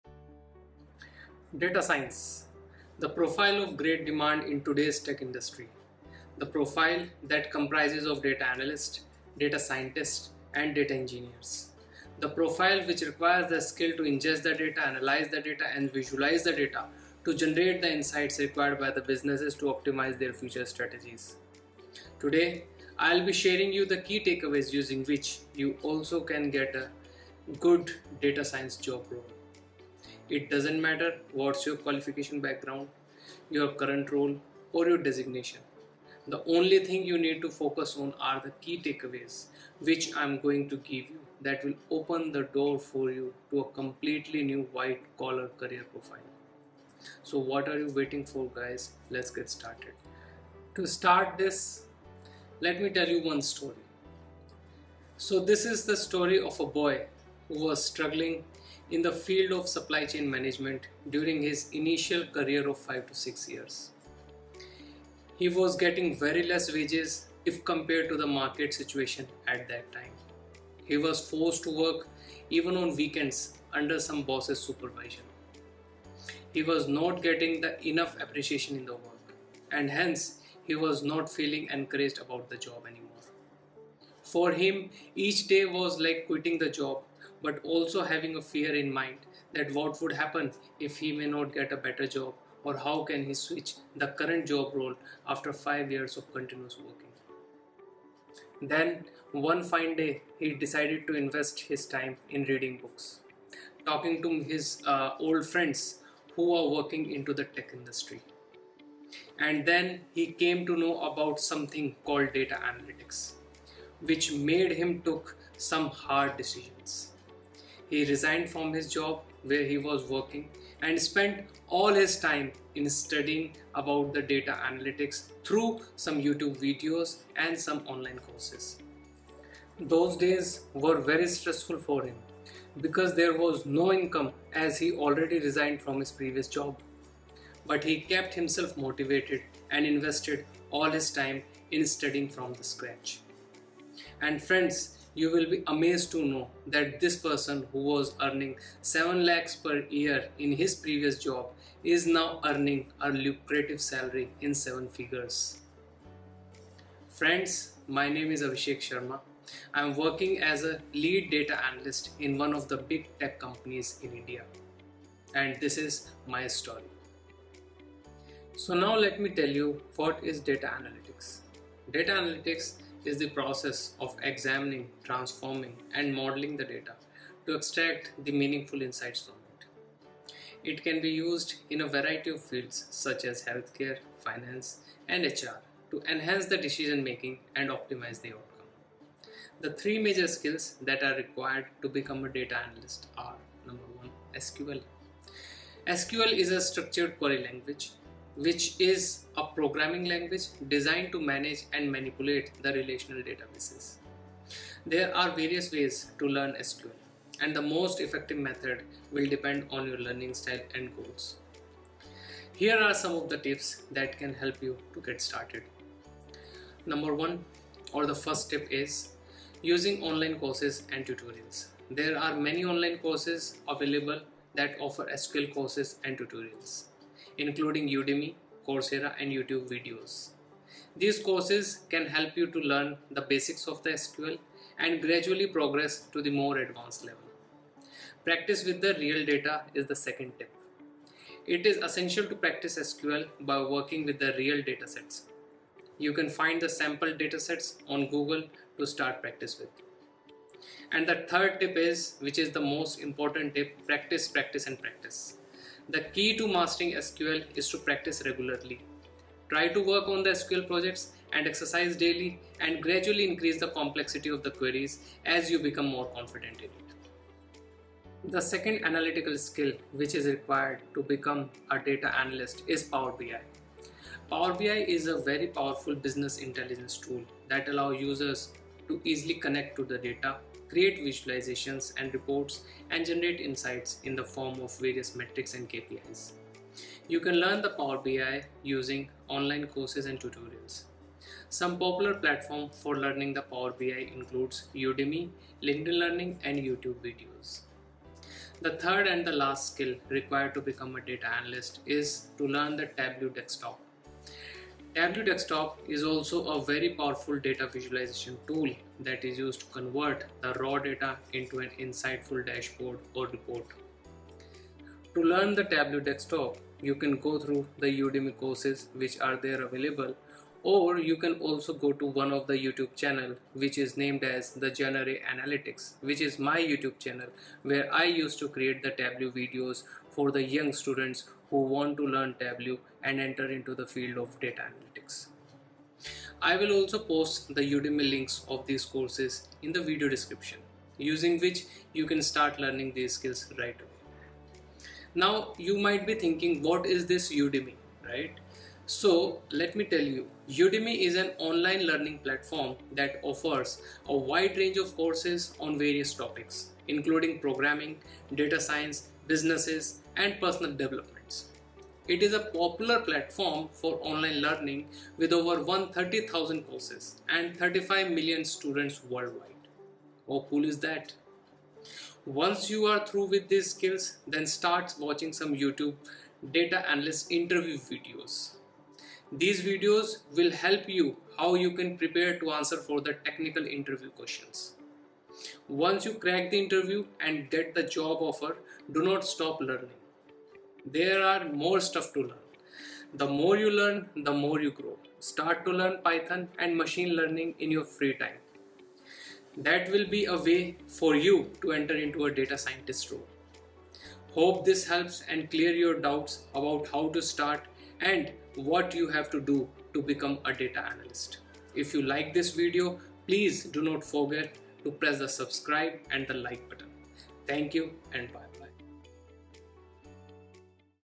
My Journey to Data Analytics | Watch this Podcast if you want to start a career in Data Science
Here I tried to explain the ways to get into the Data Analytics role. Or how to become a data analyst in 2023.

Udemy courses links:
1. For SQL course:

2. For Power BI course